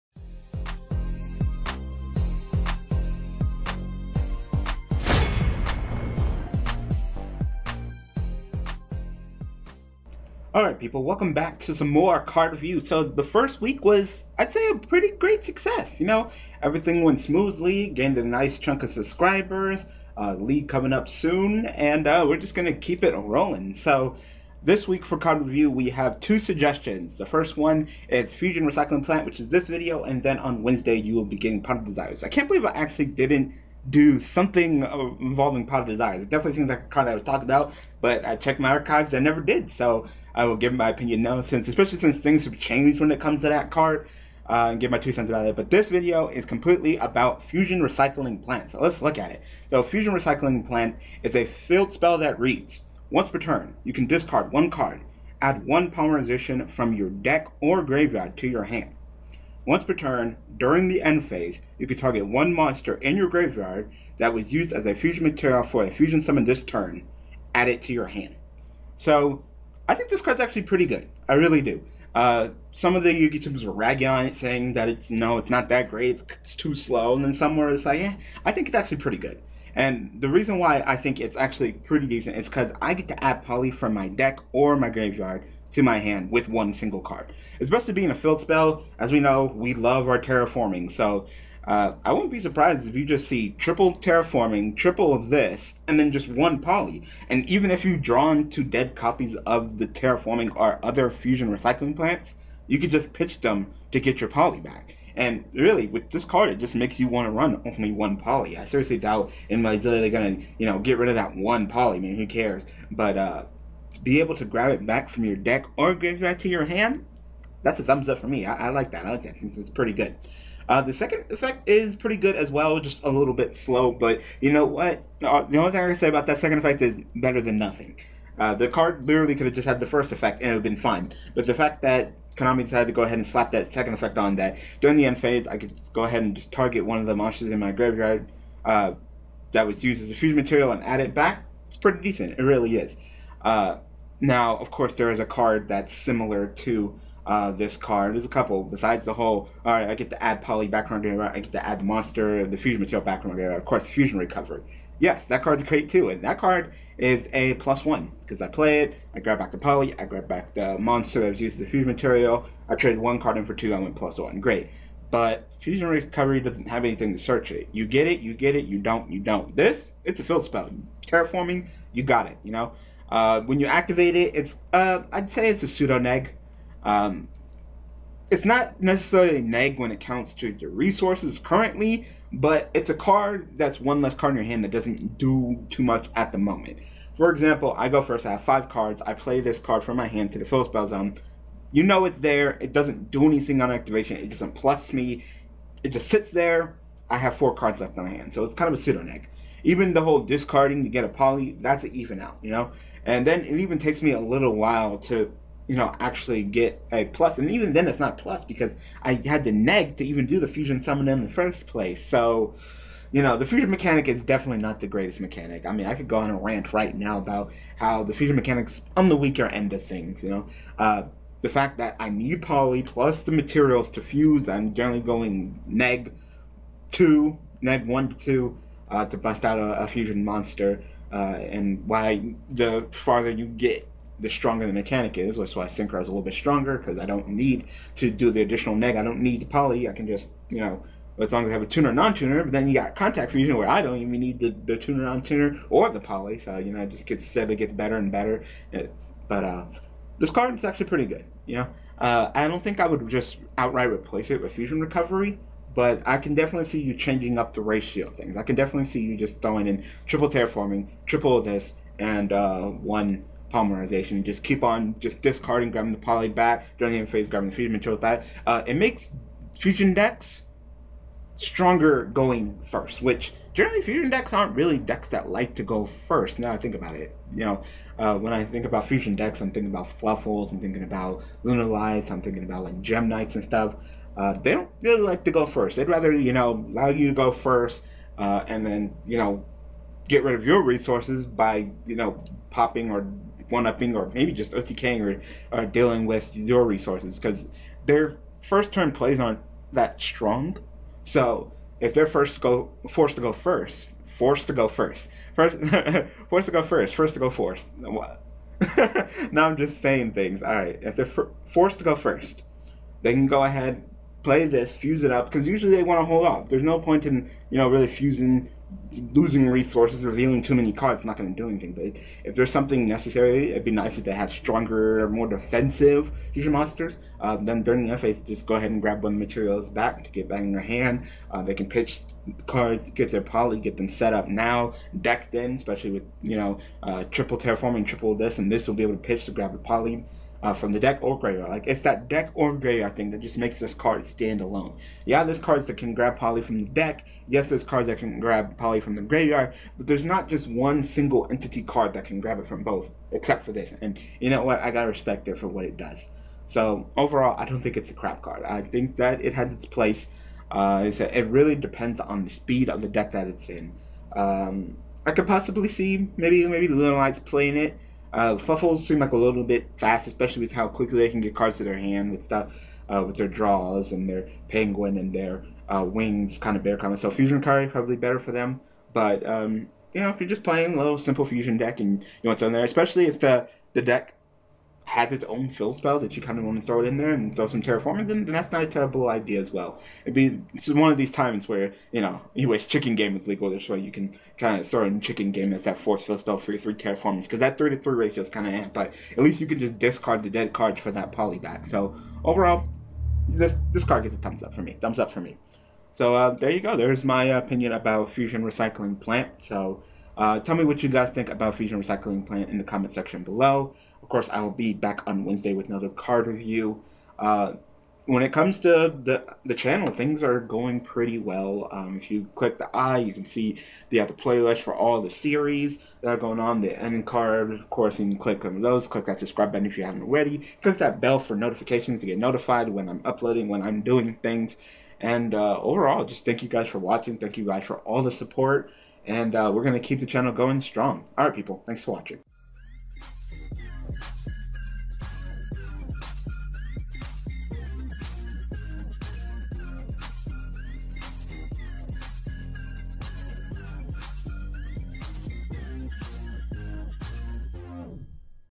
Yubel Mastery Reviews: Fusion Recycling Plant [Card Reviews]
Fusion Recycling Plant
Field Spell
Once per turn: You can discard 1 card; add 1 "Polymerization" from your Deck or Graveyard to your hand. Once per turn, during the End Phase: You can target 1 monster in your Graveyard that was used as a Fusion Material for a Fusion Summon this turn; add it to your hand.